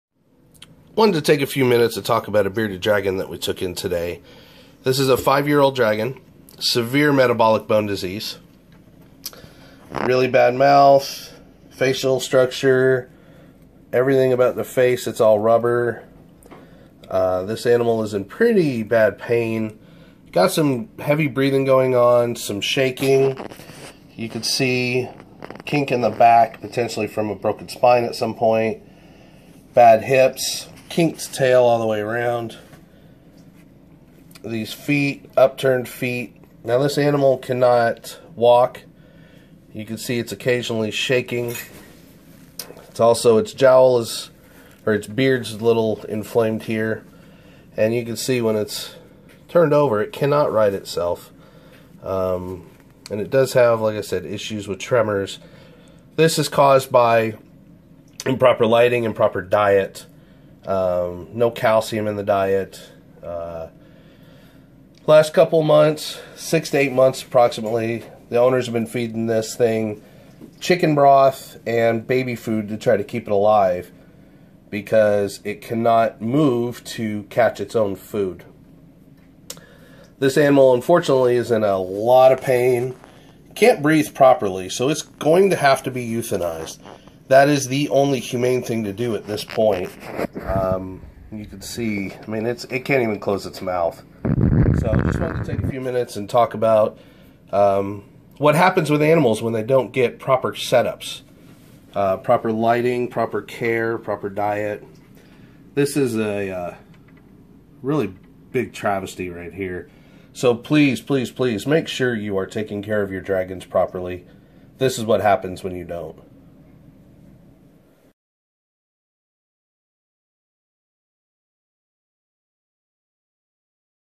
Metabolic bone disease bearded dragon
A 5 year old bearded dragon we took in today with a very severe case of MBD. This is a perfect example of why you should always do research before obtaining a pet to ensure you know ho to properly care for them.